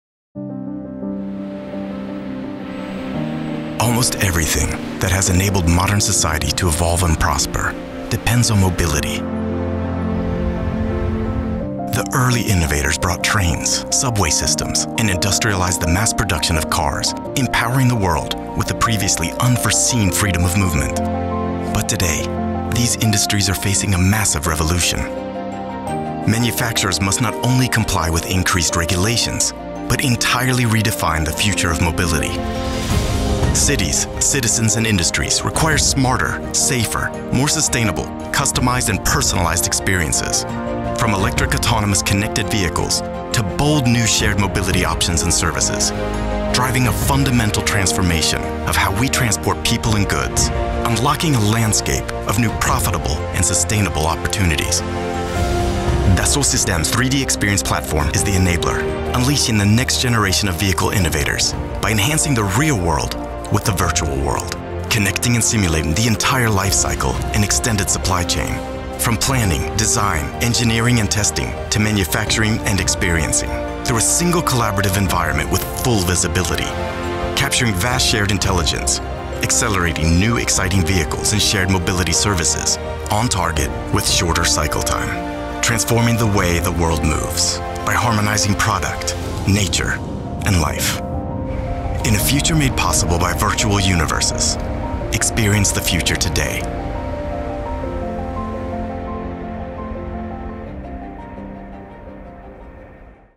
Driving vehicle innovation toward the mobility of the future - Dassault Systèmes
Almost everything that has enabled modern society to evolve and prosper depends on mobility.  The early innovators empowered the world with a previously unforeseen freedom of movement.  But today's automotive & mobility industries are facing a massive revolution.  Explore how today's OEMs and new innovators alike are entirely redefining the future of mobility with industry-leading 3DEXPERIENCE solutions.